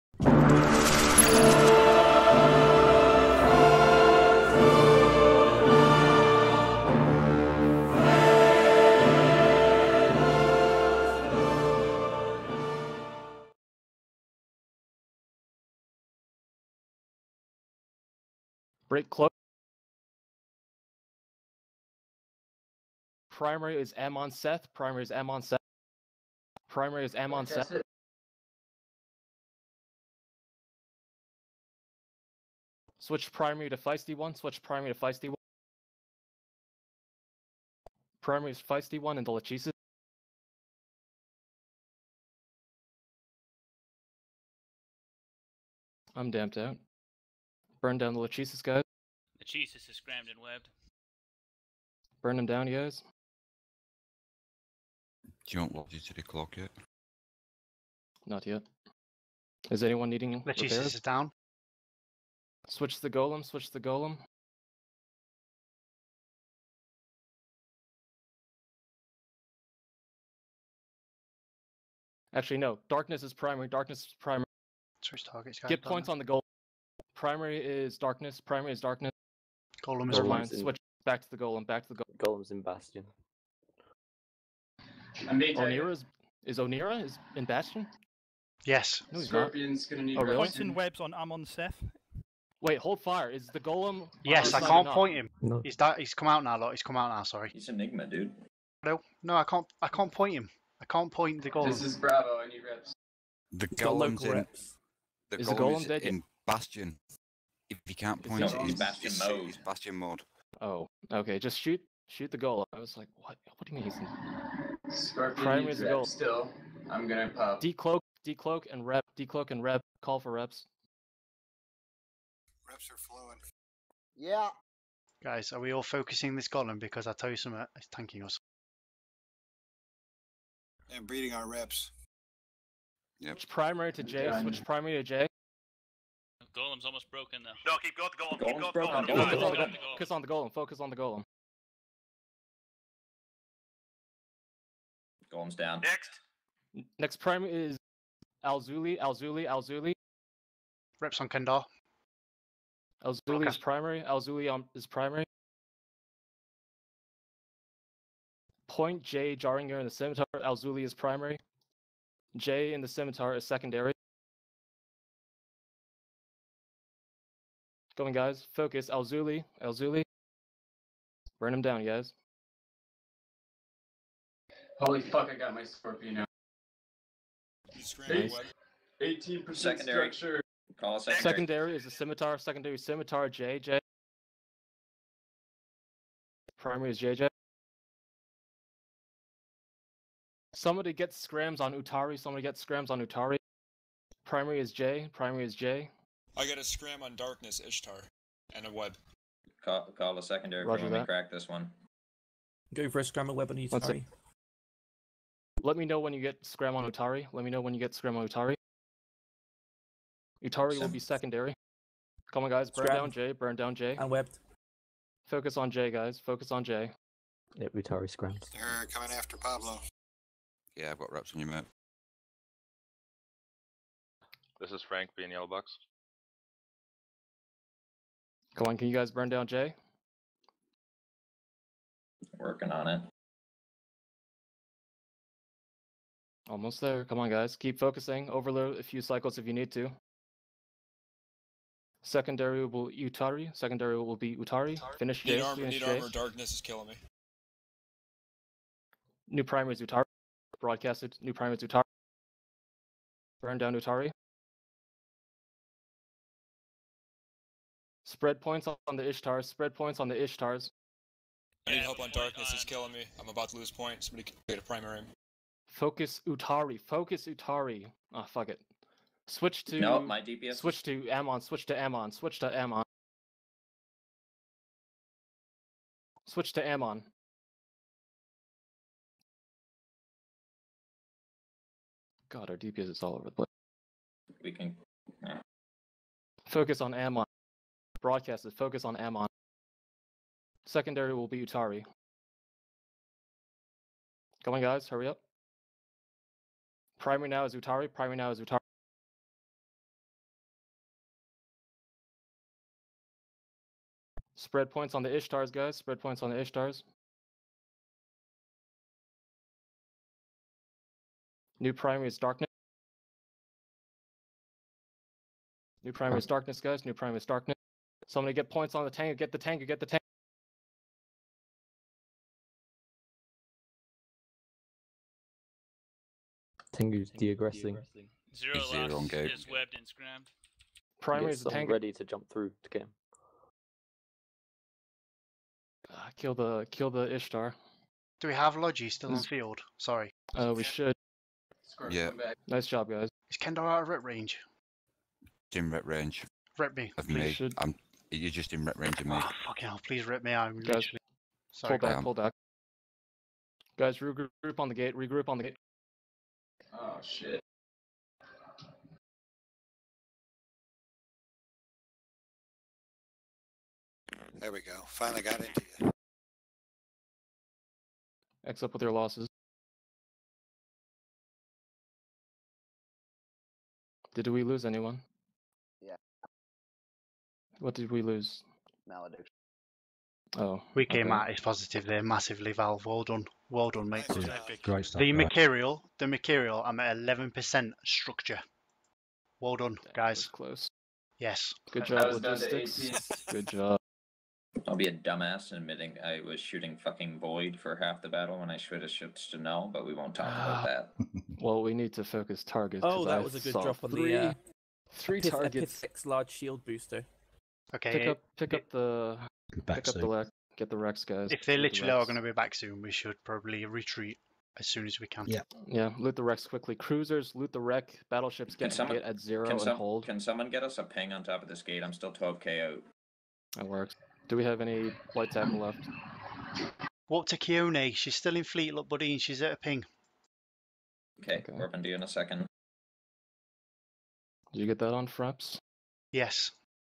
The Bastion VS Enigma Project
Story: Hostile gang in a pocket. They wanted a fight. We jumped into them.